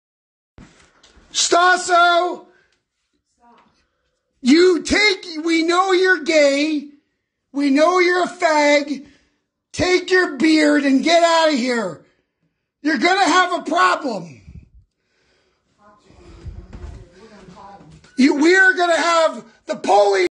The result of him pounding our wall - Part 1
This is a result of next door and his bullshit, I had enough and went off. I apologize for the use of the word "Fag", it was used in the heat of passion and I do not like that word but due to the extreme high intensity aggravation from this guy it made me just have my fill. 7/24/21.